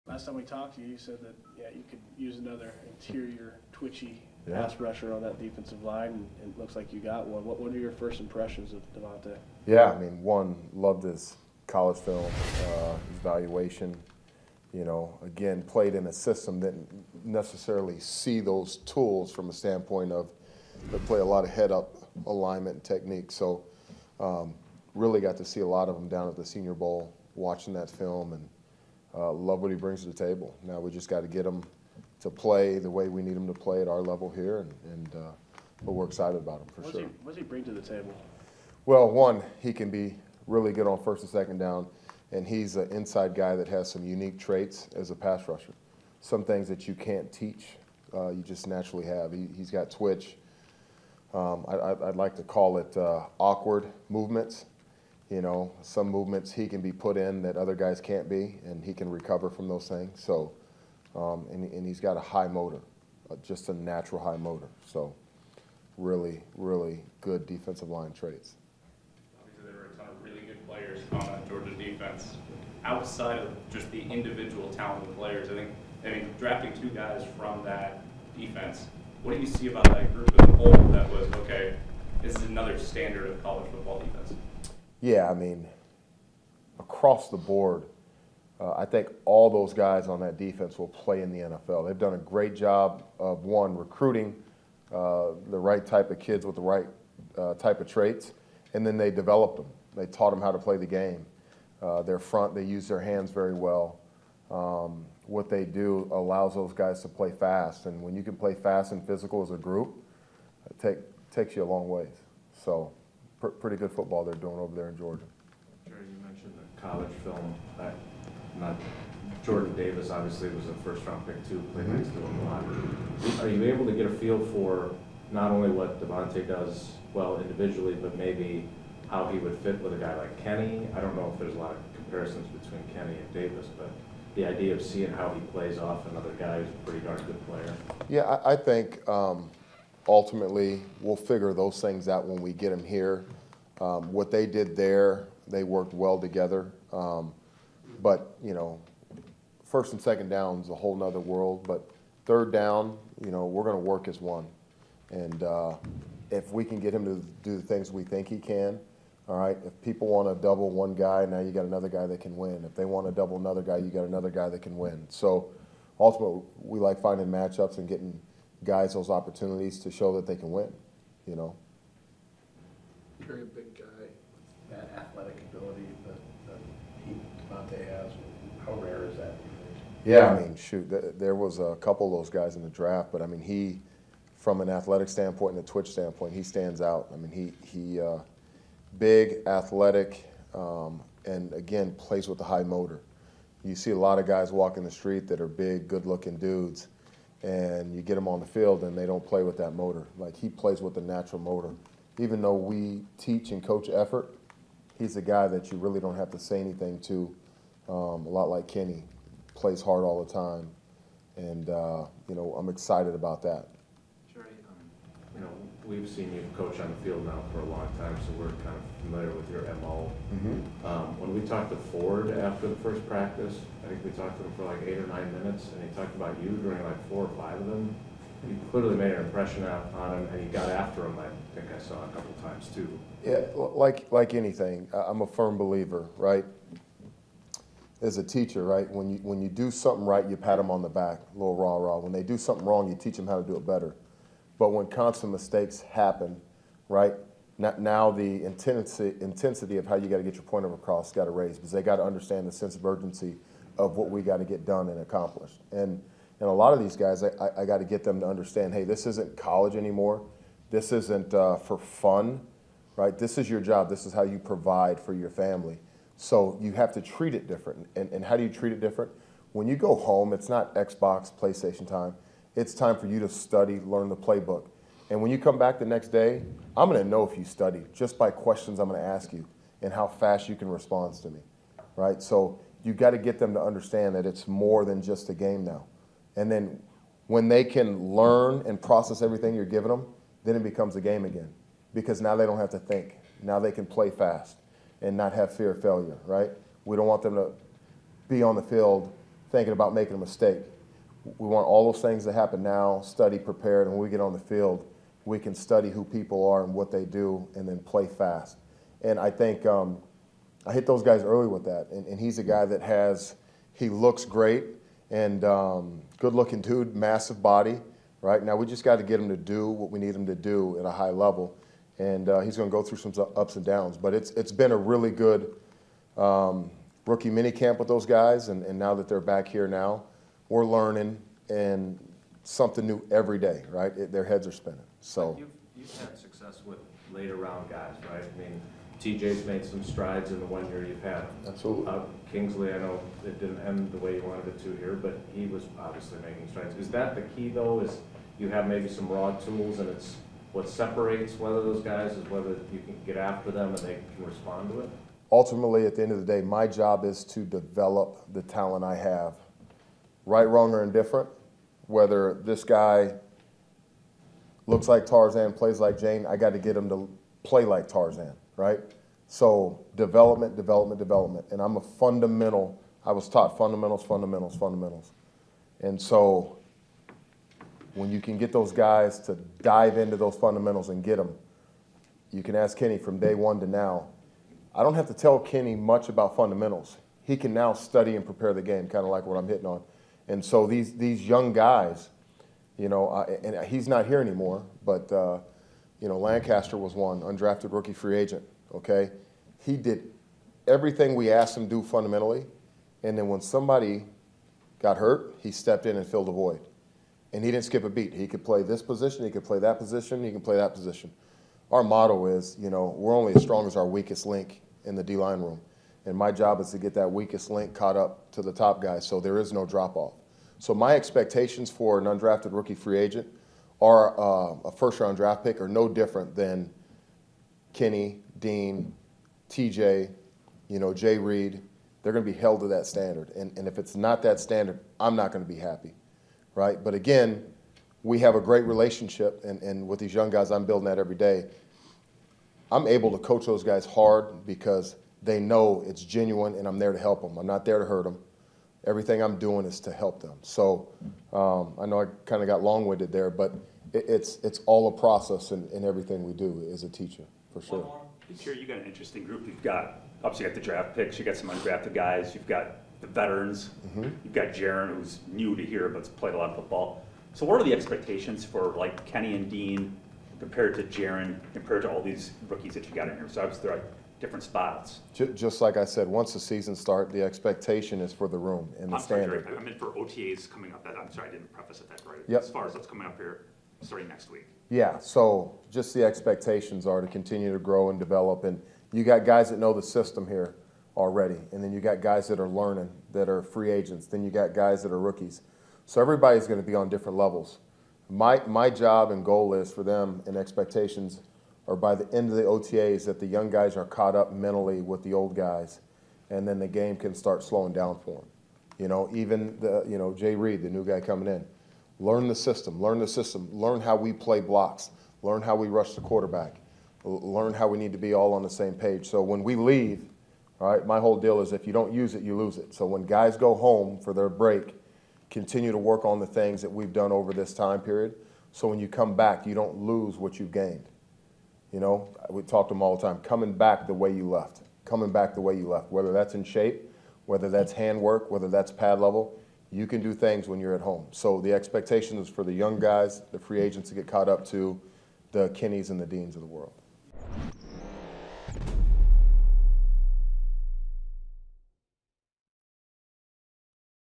Jerry Montgomery on Devonte Wyatt: 'I love what he brings to the table'
Green Bay Packers defensive line/running game coordinator Jerry Montgomery spoke to the media on Wednesday.